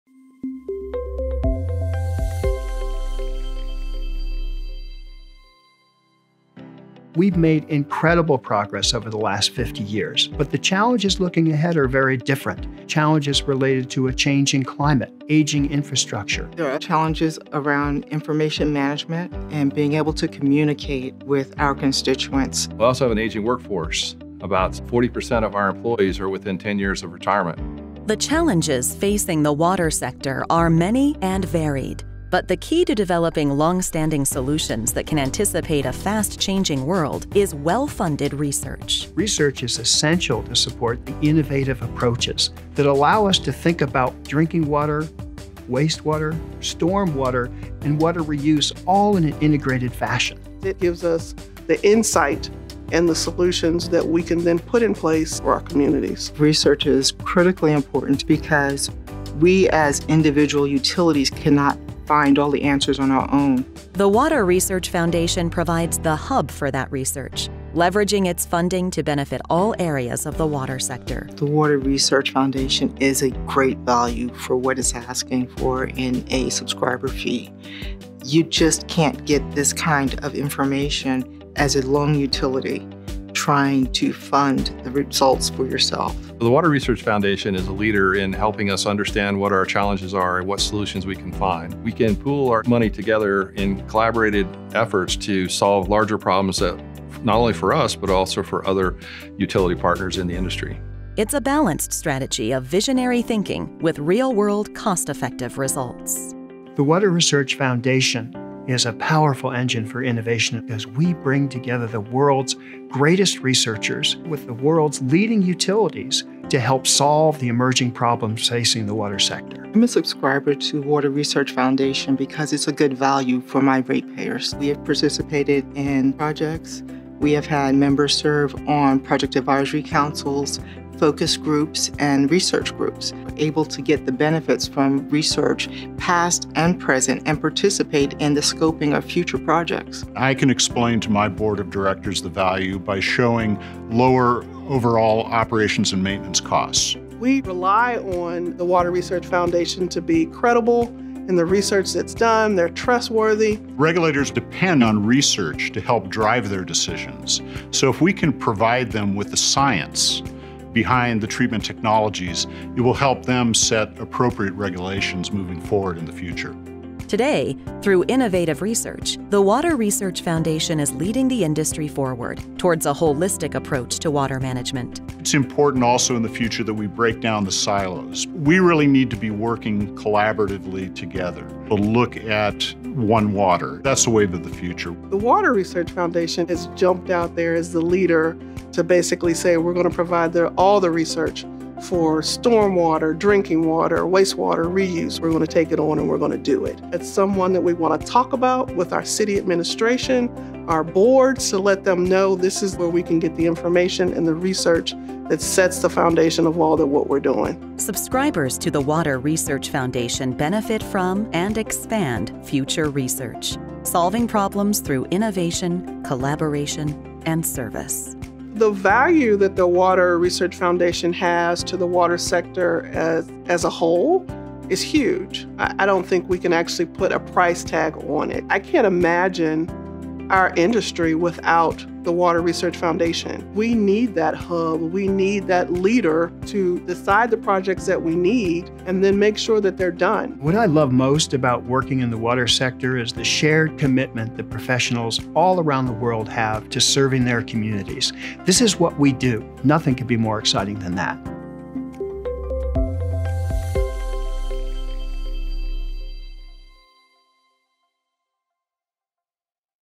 The Value of a WRF Subscription
There are many diverse challenges currently facing the water sector. Learn how well-funded research from The Water Research Foundation can help you and your organization face these challenges head-on.